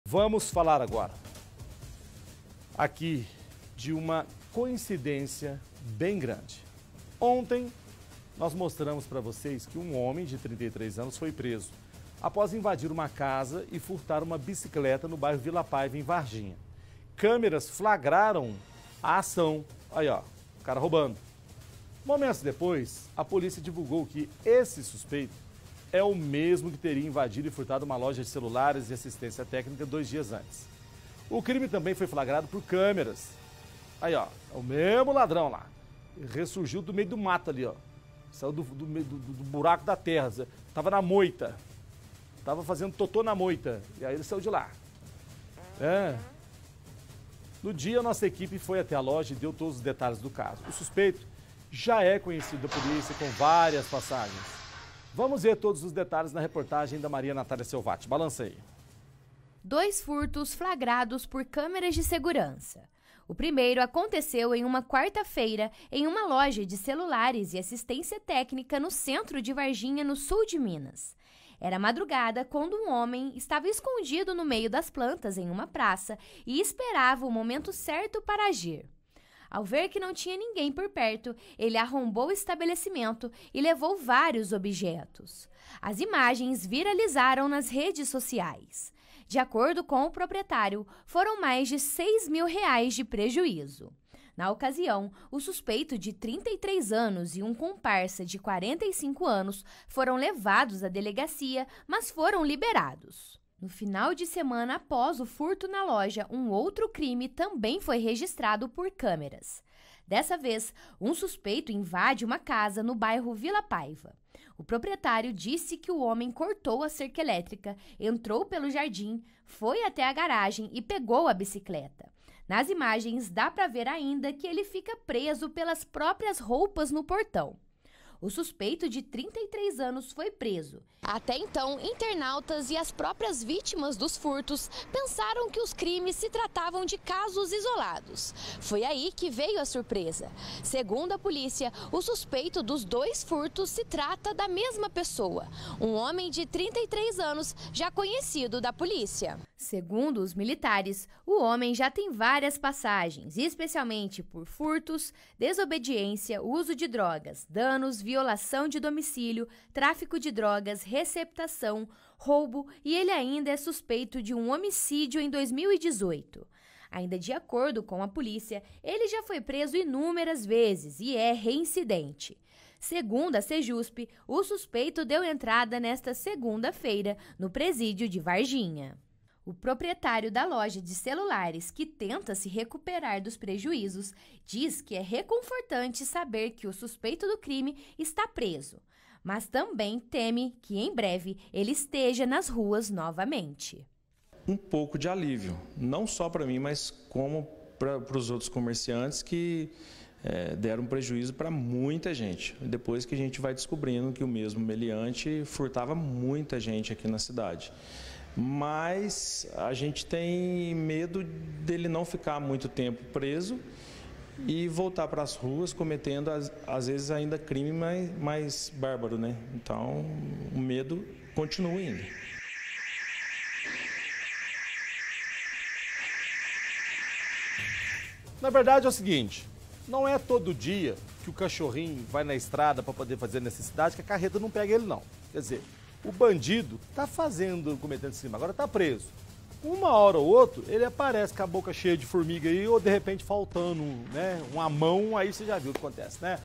ATRÁS DAS GRADES: LADRÃO DE BICICLETA E DE LOJA DE CELULARES É PRESO EM VARGINHA
Um homem, de 33 anos, foi preso em Varginha. Ele foi flagrado por câmeras de segurança invadindo uma casa para furtar uma bicicleta. Esse crime foi no bairro Vila Paiva. Segundo a PM, ele também invadiu e furtou uma loja de celulares no centro da cidade. O suspeito é muito conhecido no meio policial e acumula diversas passagens, incluindo um homicídio em 2018.